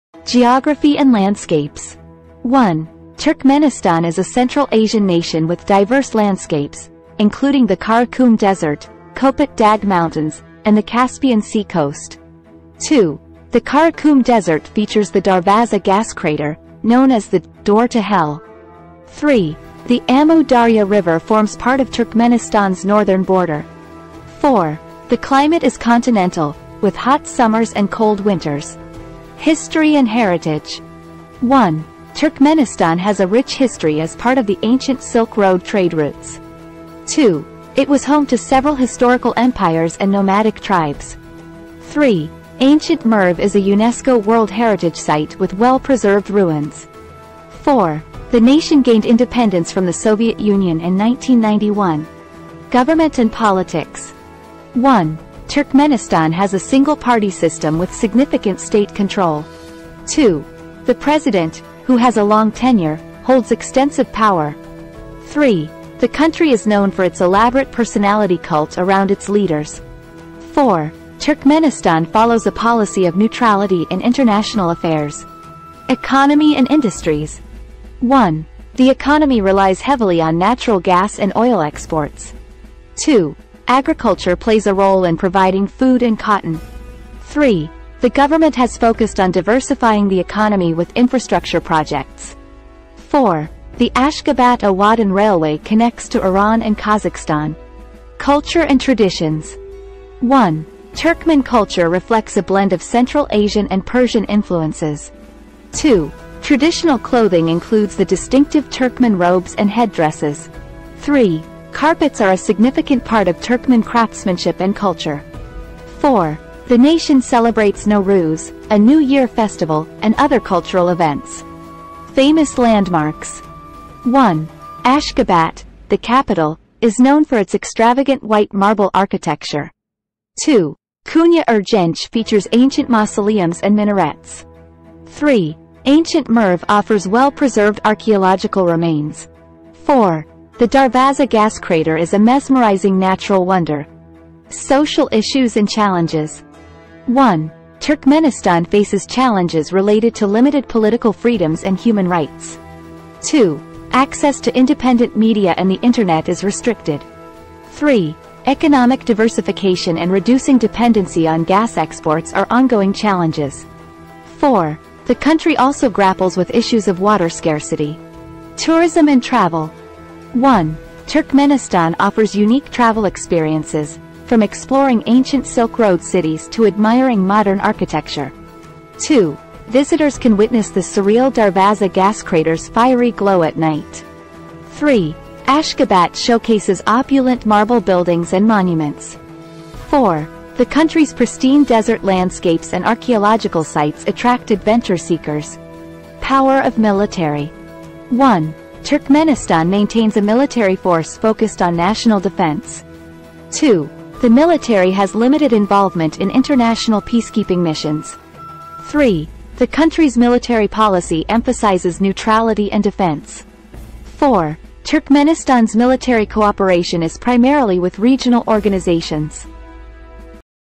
Turkmenistan: Unveiling the Secrets of the Silk Road Oasis
Join us on a captivating journey through Turkmenistan, a hidden gem along the Silk Road, where ancient history meets futuristic ambitions. In this video, we'll explore Turkmenistan's stunning geography, uncover its millennia-old history and heritage, navigate its unique government and politics, delve into its evolving economy and industries, immerse ourselves in its rich culture and traditions, visit iconic landmarks, address pressing social issues, and understand the power of its military. Discover the essence of Turkmenistan, where the past and future coexist in harmony.